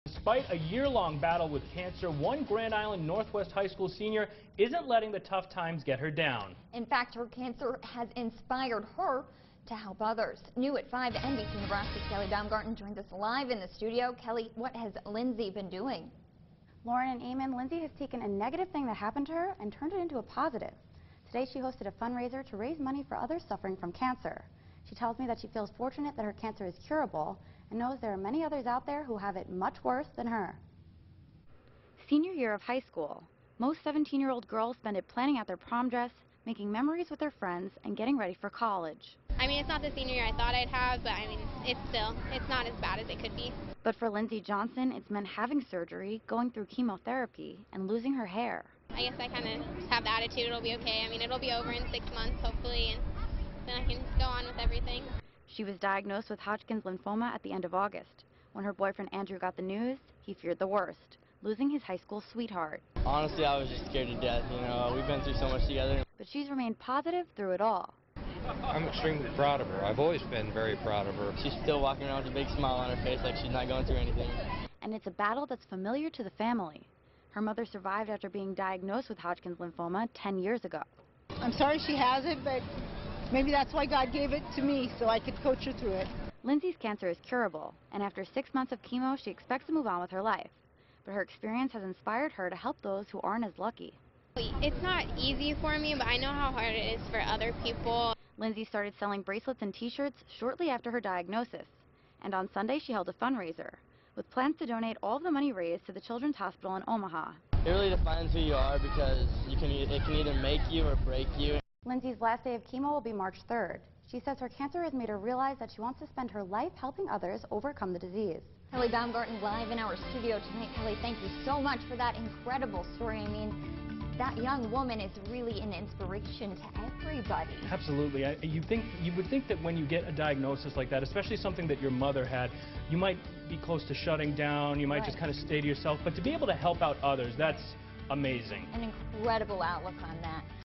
Grand Island Senior Suffering from Cancer is Inspired to Help Others / October 12, 2014
A 17 year old Grand Island girl has been battling cancer since August, but the Northwest high school senior hasn’t let the tough times get her down. In fact, her cancer has inspired her to help others.

Lindsey has taken a negative thing that happened to her and turned it into a positive. Today she hosted a fundraiser to raise money for others suffering from cancer. She tells me she feels fortunate that her cancer is curable and knows there are many others out there who have it much worse than her.

Senior year of high school. Most 17 year old girls spend it planning out their prom dress, making memories with their friends, and getting ready for college.

“It’s not the senior year I thought I’d have but it’s not as bad as it could be," said Lindsey Johnson.

But for Lindsey Johnson it’s meant having surgery, going through chemo therapy, and losing her hair.

“I have the attitude that it’ll be okay, it’ll be over in six months hopefully and then I can move on," said Lindsey.

She was diagnosed with Hodgkins Lymphoma at the end of August.

“Honestly I was scared to death we’ve been through so much together," said Andrew Rojas, Lindsey's boyfriend.

But she’s remained positive throughout it all.

“I’m extremely proud of her I’ve always been proud of her but how she’s handling this and not being selfish and never going through a period of why is this happening to me or feeling sorry for herself," said Brent Johnson, Lindsey's father.

“She’s still walking around with a big smile on her face like she’s not going through anything," said Andrew.

It's a fight that's familiar to the family. Her mother survived after being diagnosed with Hodgkins Lymphoma 10 years ago.

“I’m sorry she has but maybe that's why god gave it to me so i could coach her through it," said Lindsey's mother, Diane Johnson.

Lindsey’s cancer is curable, and after 6 months of chemo she expects to move on with her life, but her experience has inspired her to help those who aren’t as lucky.

“It’s not easy for me but i know how hard it is for other people," said Lindsey.

Lindsey starts selling bracelets and t-shirts shortly after her diagnosis, and on Sunday she held a fundraiser. She plans to donate all of the money raised to the Children’s Hospital in Omaha.

“It really defines who you are because it can either make you or break you but she has her held up high and she’s like I’m gonna get through this," said Andrew.

Lindsey’s last day of chemo will be March 3rd. She says her cancer has made her realize that she wants to spend her life helping others overcome the disease.